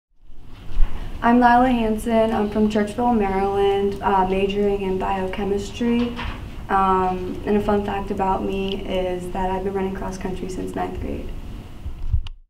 Laila Hanson
Meet Laila Hanson, a member of the Cross Country team!